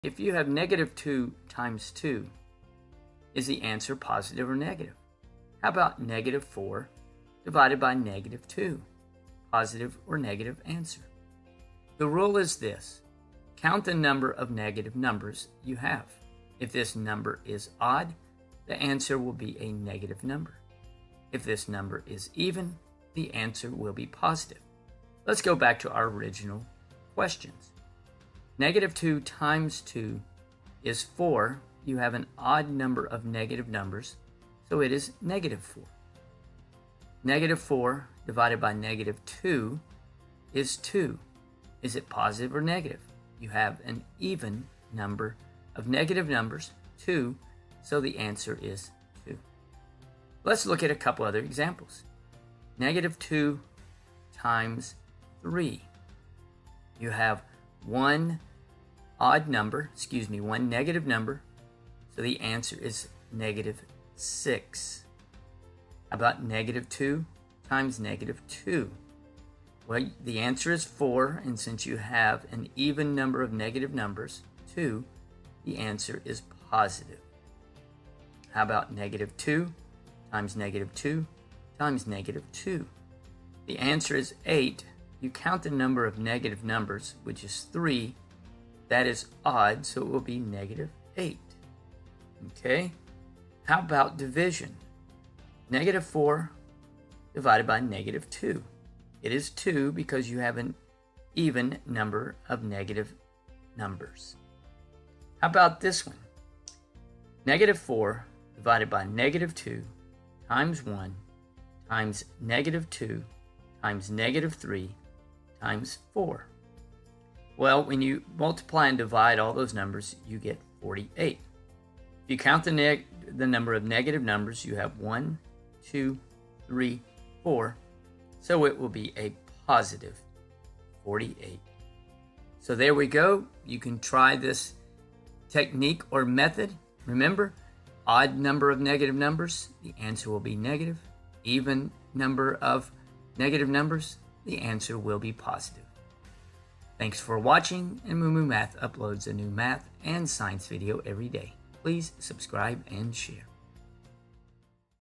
Rules for Multiplying and Dividing POSITIVE and NEGATIVE numbers
Whenever you multiply or divide a positive and negative number it can be confusing if the answer is positive or negative. For example, -5 x -4 x 2.Is the answer positive or negative.

Here is a simple math trick to make this math operation easy.
Just count the number of negative numbers you have.
If you have an odd number then the answer is negative.
If you have an even number then the answer is positive.
For example 2 x -2 x 1 x -1 = ??

Count the number of negative numbers.
You have an even number ( 2 ) so the answer is positive.